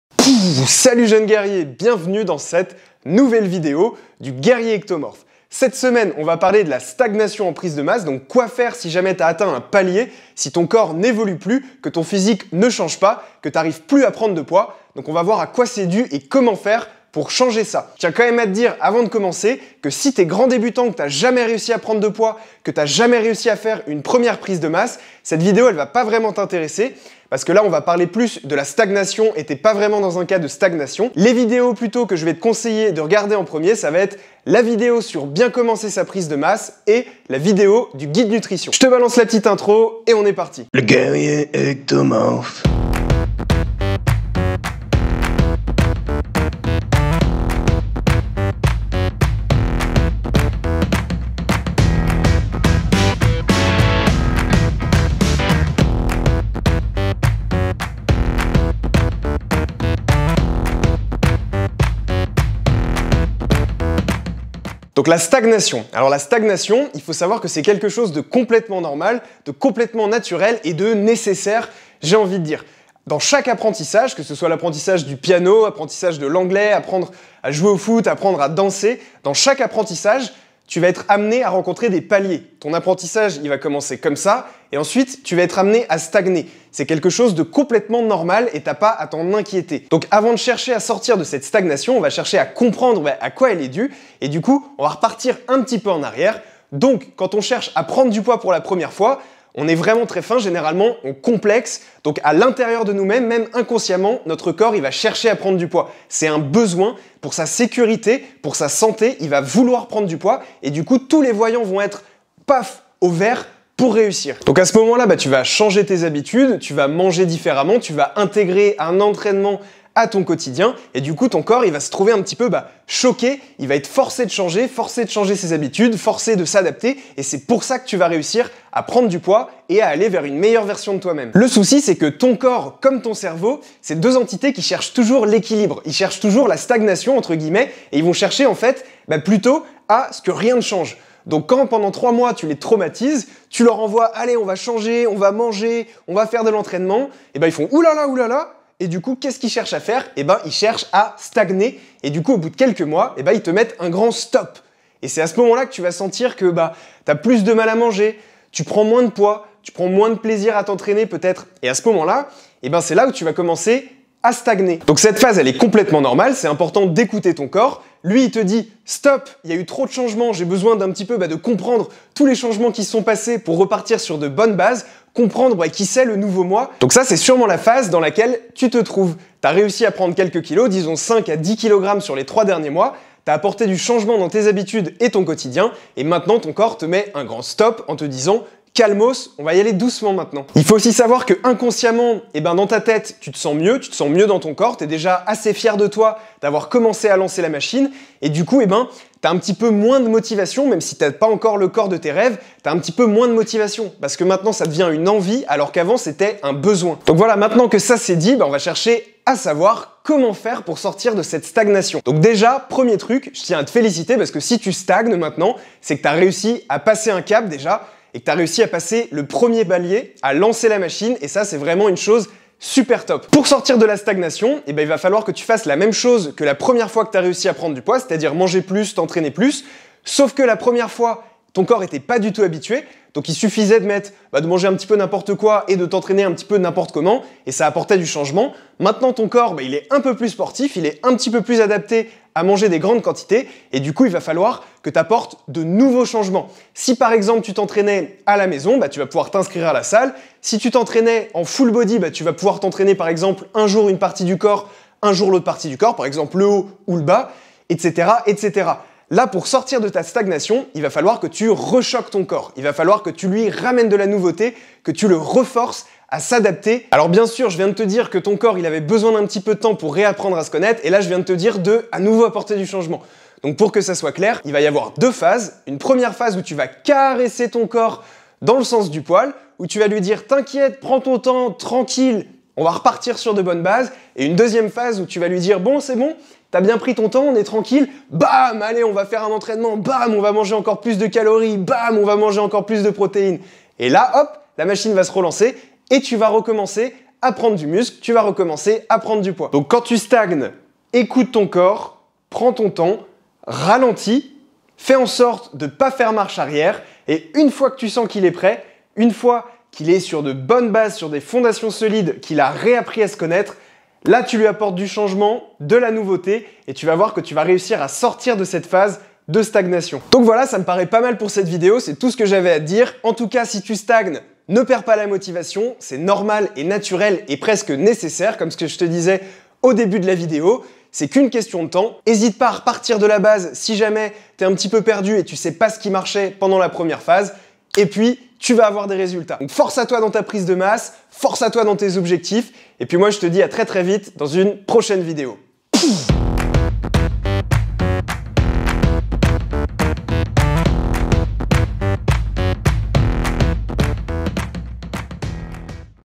Je n'arrive plus à prendre de poids : Que Faire ? (Stagnation Prise de Masse)
Salut Jeune Guerrier ! ⚔️
Dans cette vidéo je te parle de la stagnation en Prise de Masse.

C'est tout à fait normal parfois d'avoir des moments de mous, des moments où tu n'arrives plus à prendre du poids et je t'explique à quoi c'est dû et comment faire pour en sortir dans cette vidéo.

1:06 - Comprendre d'où vient la Stagnation
3:26 - Comment faire pour sortir de la Stagnation ?
4:59 - Récap de la vidéo + Conclusion

▬▬▬▬▬▬▬▬▬ LA MISSION DU GUERRIER ⚔️ ▬▬▬▬▬▬▬▬▬

Aider toutes les personnes qui galèrent à prendre du poids à y arriver, si c'est ton cas

▬▬▬▬▬▬▬▬▬ LES VIDEOS IMPORTANTES 📹 ▬▬▬▬▬▬▬▬▬

▬▬▬▬▬▬▬▬▬ LES ARTICLES IMPORTANTS 📖▬▬▬▬▬▬▬▬▬